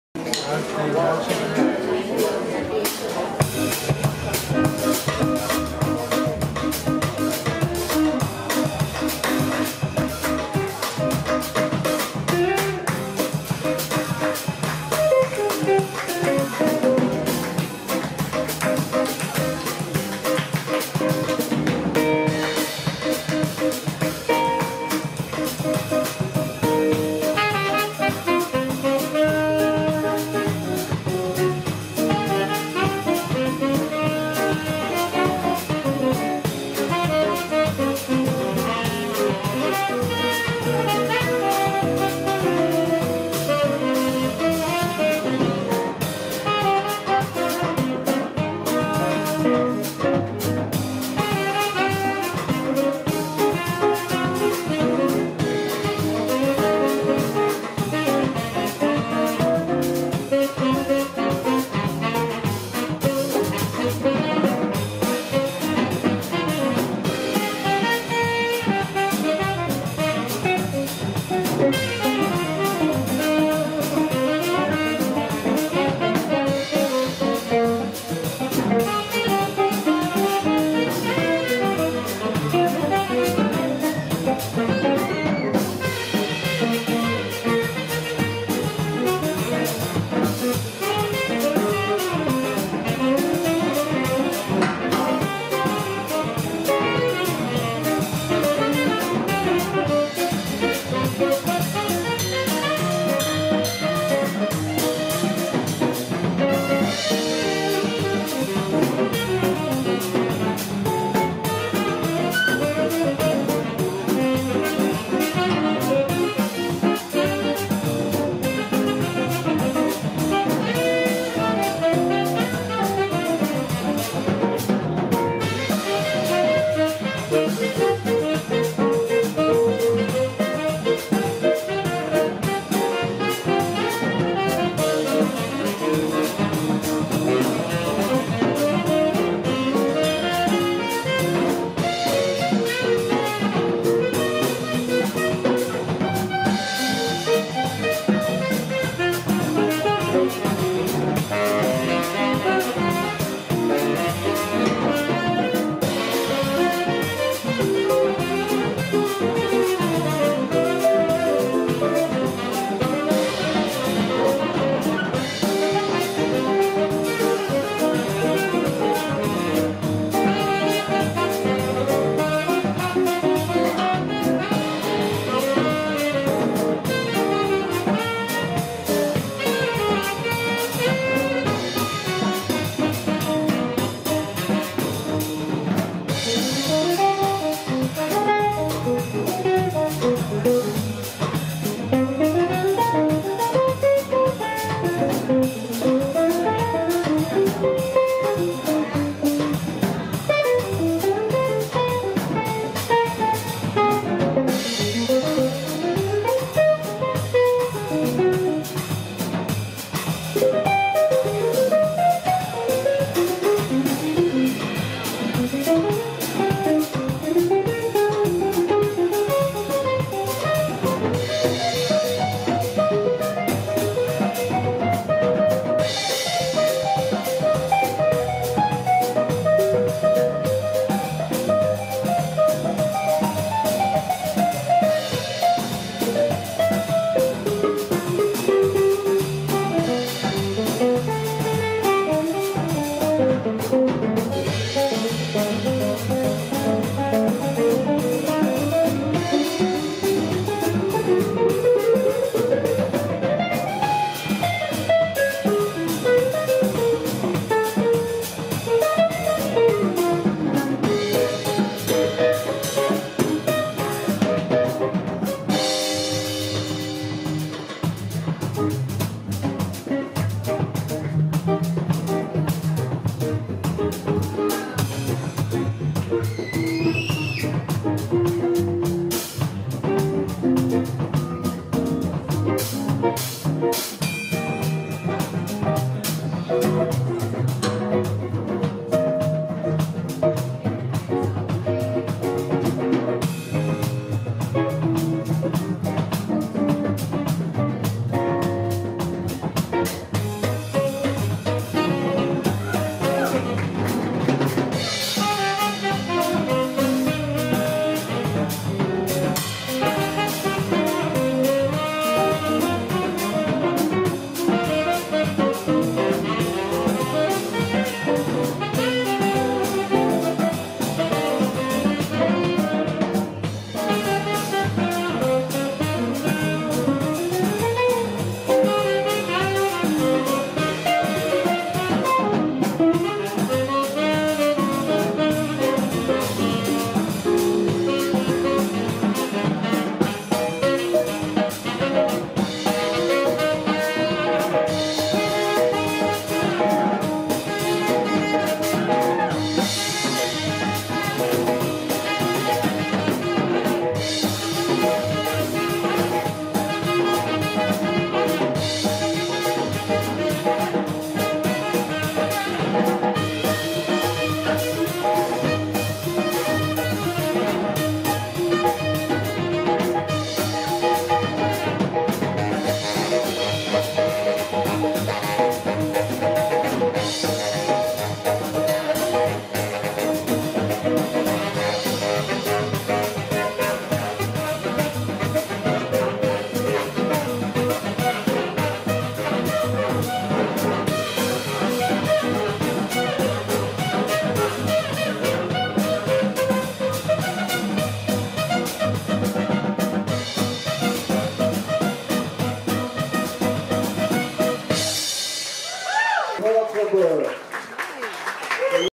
Samba! +Blackbird3 Wild_Austin_music...... (with dancers!)
A bit of fun, from the 24th November, up on the porch, Central Market Music.
Cheers!